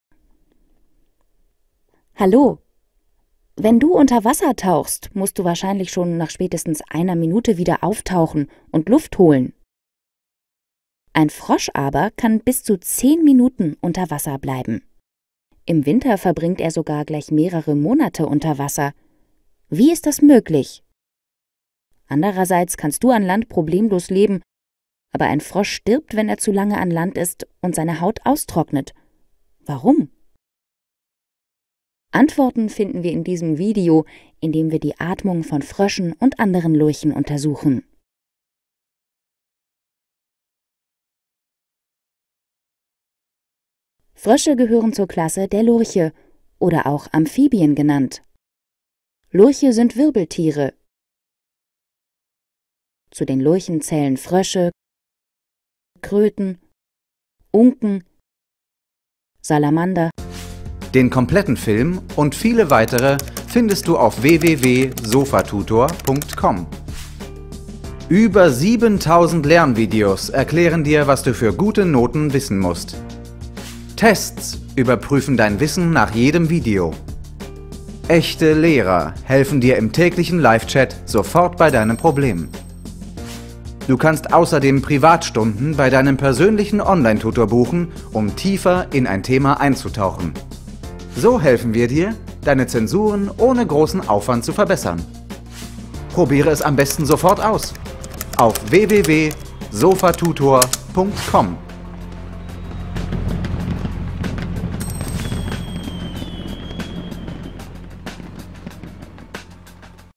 Atmung bei Lurchen | Biologie | Tiere
Dieses Video zeigt dir, wie die Atmung bei Lurchen funktioniert. Sie verfügen sowohl über ein Lungenatmung als auch eine Hautatmung. Mehr dazu in diesem Film.

Dieses Video gehört zu: Biologie | Tiere | Fische und Lurche